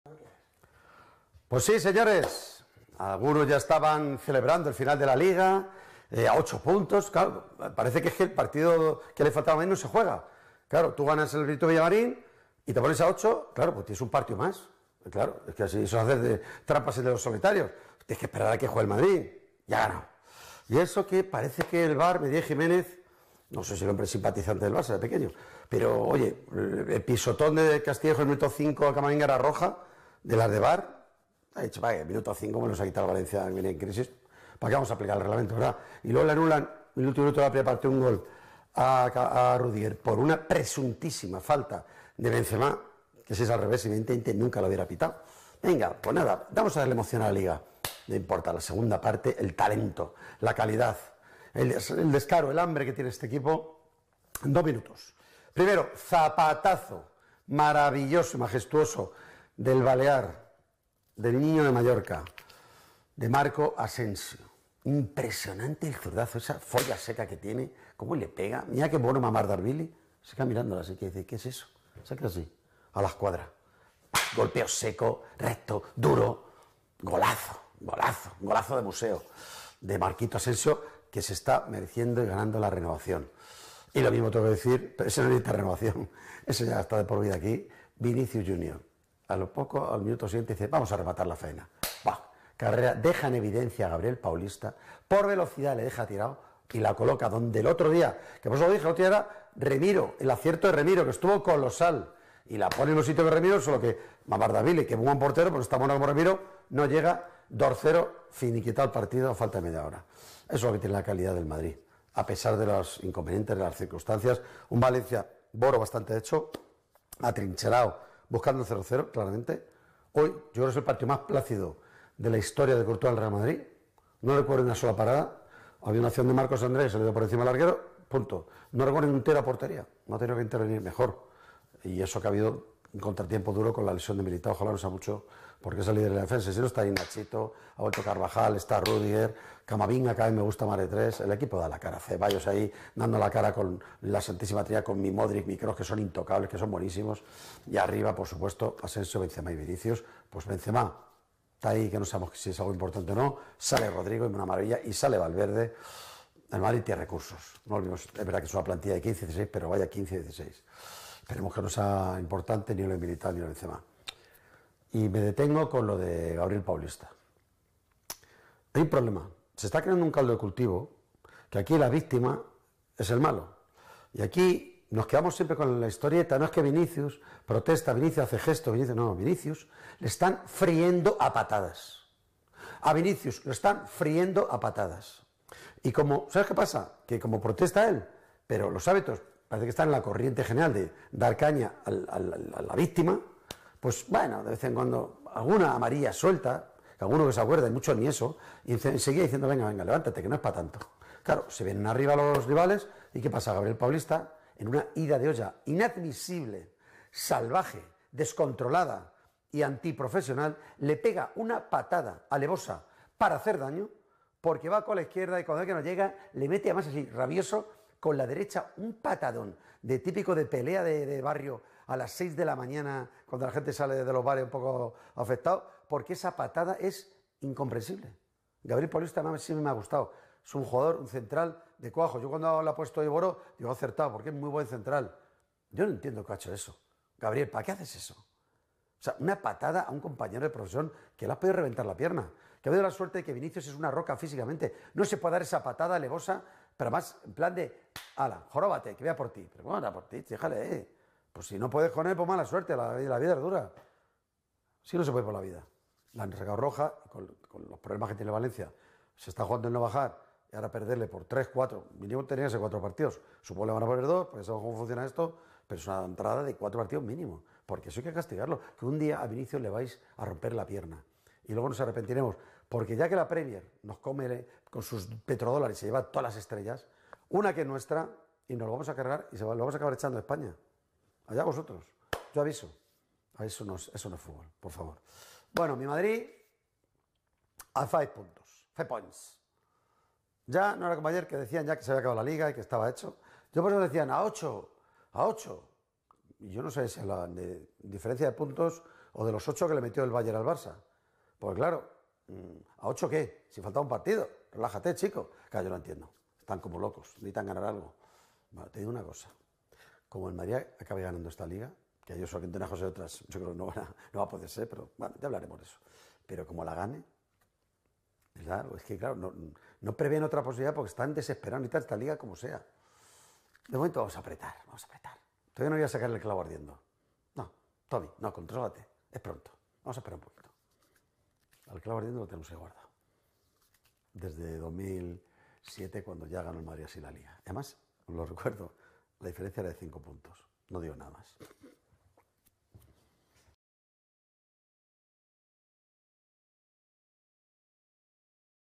REAL MADRID 2- VALENCIA 0 | El DISCURSO de RONCERO: indignación por el PATADÓN sobre VINICIUS | AS
Roncero sacó pecho tras la victoria de los blancos que les mantiene en la pugna por LaLiga. Se dirigió directamente a Paulista por su agresión a Vinicius y dejó un mensaje que hay que escuchar.